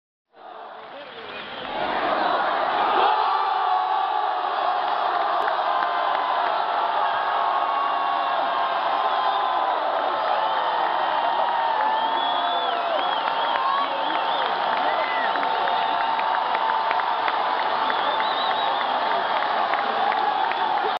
Mundial Sub-17:Mexico 2-Panama 0(Segundo gol de México)
Segundo Gol de México correspondiente al duelo frente a Panama desde las tribunas del Estadio Hidalgo.